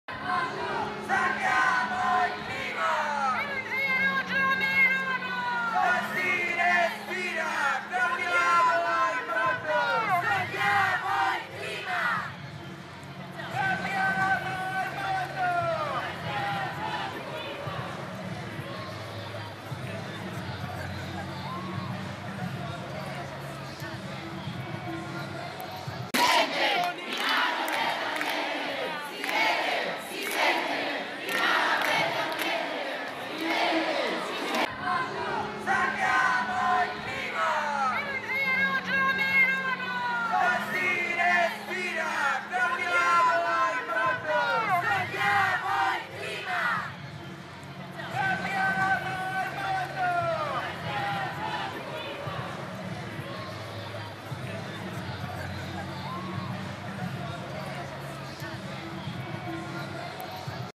Global Strike, 30mila giovani in marcia per il clima a Milano
(Agenzia Vista) - Milano, 24 Maggio 2019 - Da Largo Cairoli è partita la manifestazione per il clima, simbolo della protesta la giovane Greta Thunberg, che ha riportato all’attenzione del pubblico la tematica ambientale. Courtesy Instagram Ig frensis.a